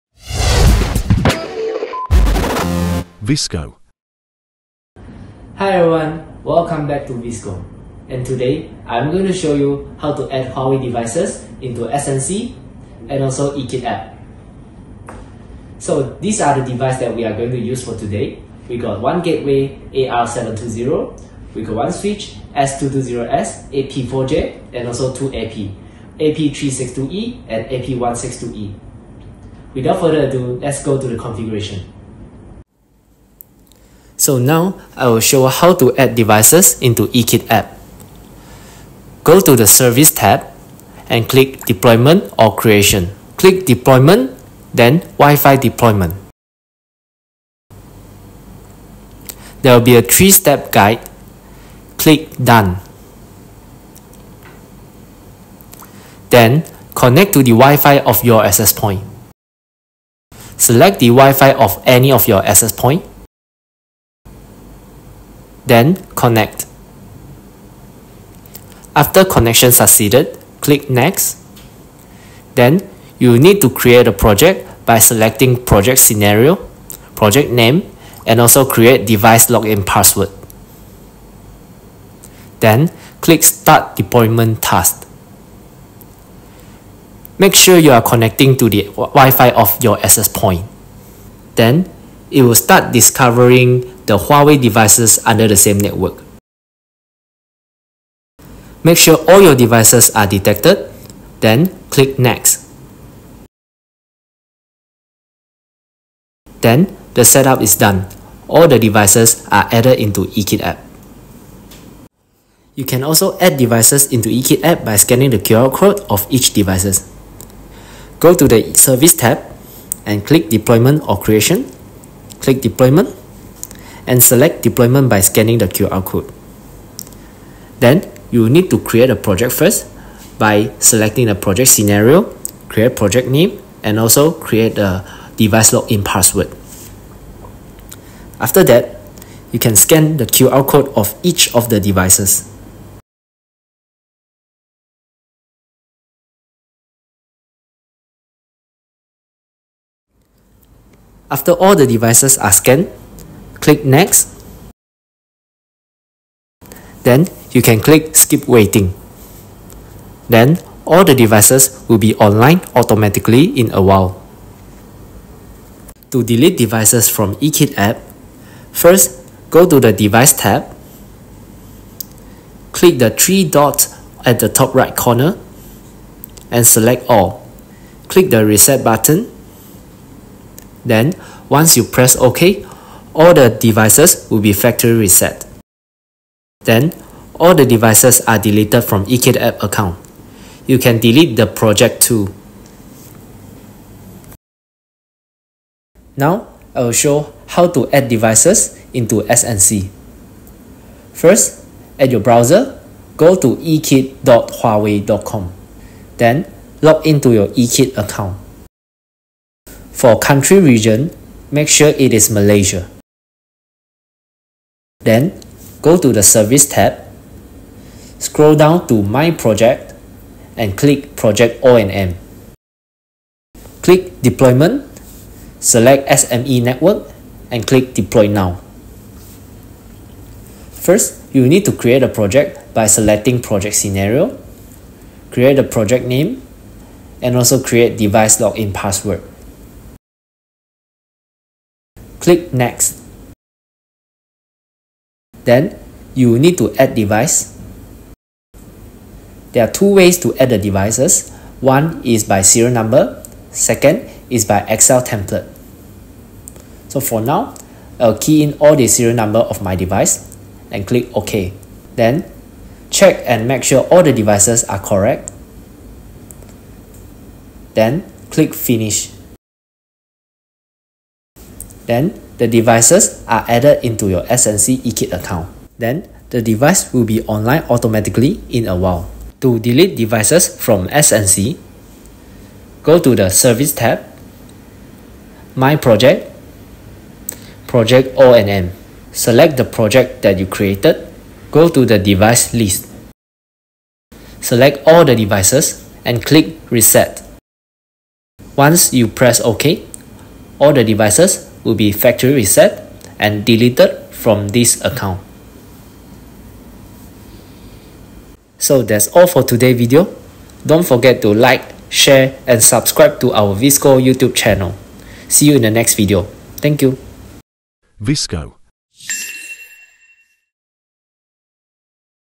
HOW TO : Add & Delete Devices from Huawei eKit App and SNC Web
Need to manage devices in eKit & SNC? This guide walks you through the process of adding and deleting devices with ease!